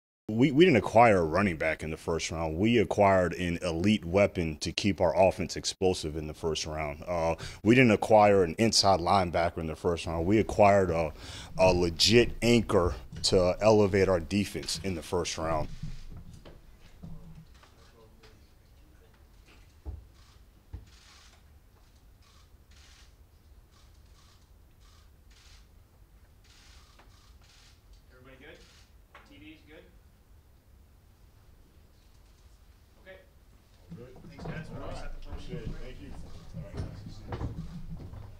Lions GM Brad Holmes defends first round selections of Jahmyr Gibbs and Jack Campbell
On Jahmyr Gibbs: "We didn't acquire a running back in the first round. We acquired an elite weapon to keep our offense explosive."

On Jack Campbell: "We acquired a legit anchor to elevate our defense."